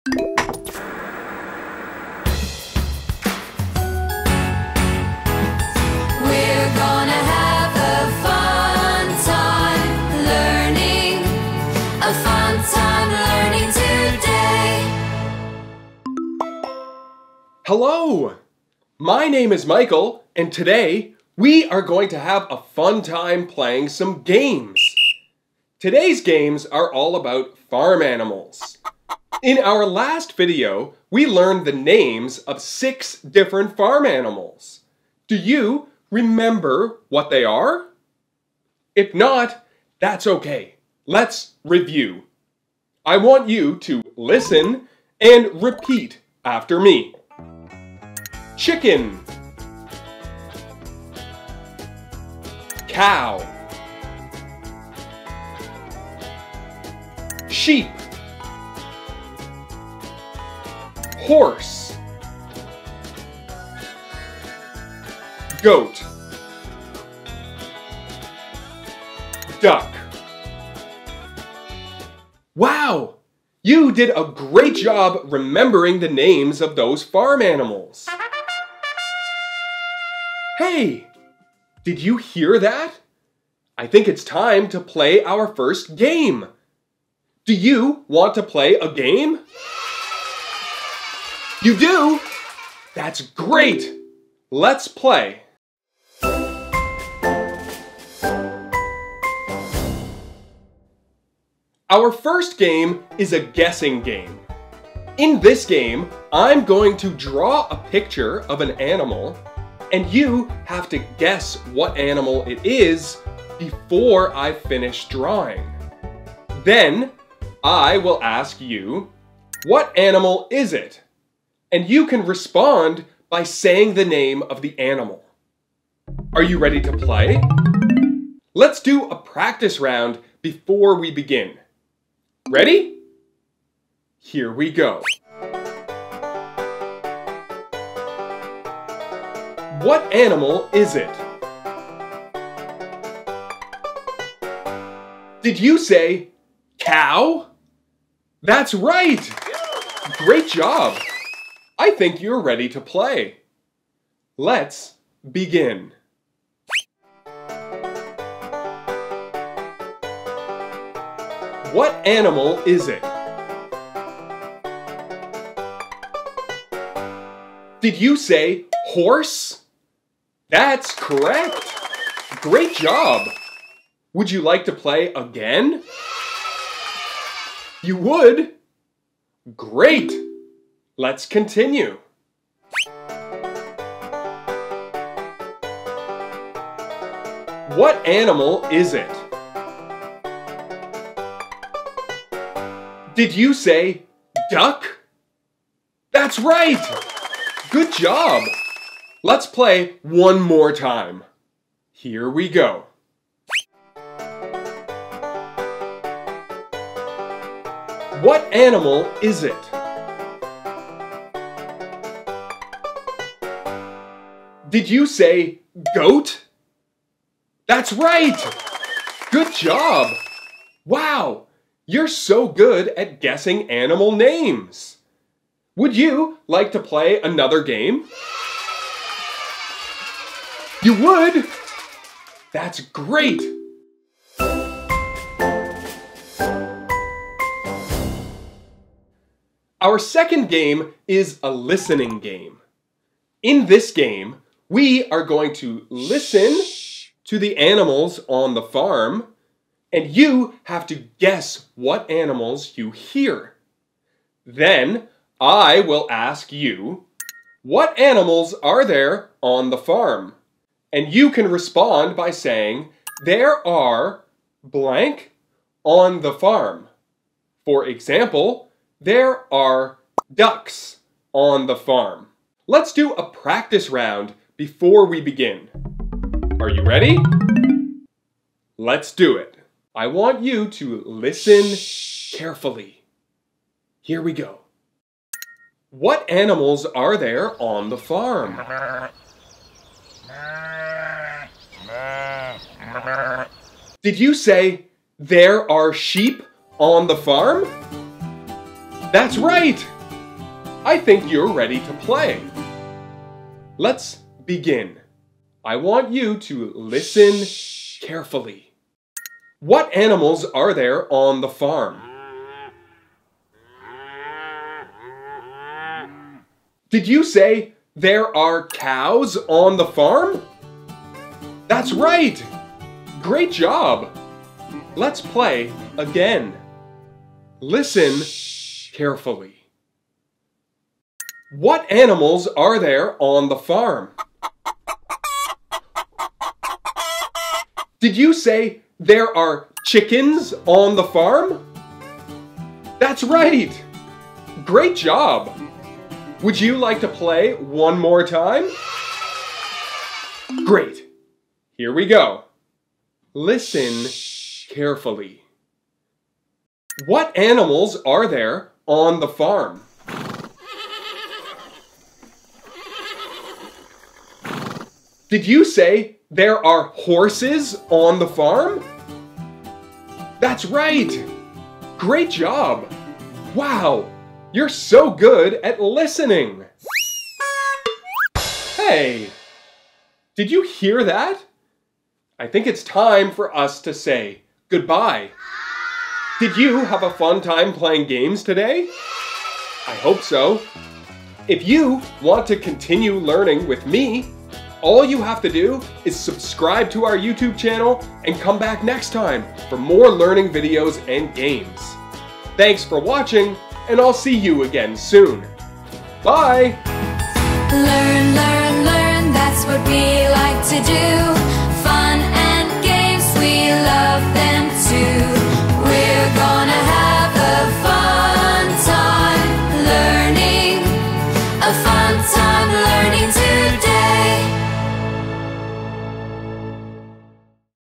Farm Animal SOUNDS and DRAWING GAME || Easy English Activities for Kids
Today we’re going to play two games that will help reinforce the vocabulary in our last video about farm animals.

GAME 1

The first game today is a vocabulary guessing game. Students will watch a drawing of an animal take shape and guess what animal it is before the drawing is complete.

For this game, students will need to be familiar with the following farm animals:

CHICKEN
COW
SHEEP
HORSE
GOAT
DUCK

GAME 2
Our second game is a listening game. In this game, students will need to identify the animal they hear based on the sound it makes. During this game, students will be asked to answer the question, “What animals are there on the farm?”

For example:
“What animals are there on the farm?”
“There are cows on the farm.”

If you’d like to skip to a specific game you can do so using the timestamps below:

TIMESTAMPS:
0:00 - Hello!
0:40 - Review (chicken, cow, sheep, horse, goat, duck)
1:36 - Game #1 (Guess the Animal Drawing Game)
3:53 - Game #2 (Guess the Animal Listening Game)
6:17 - Goodbye!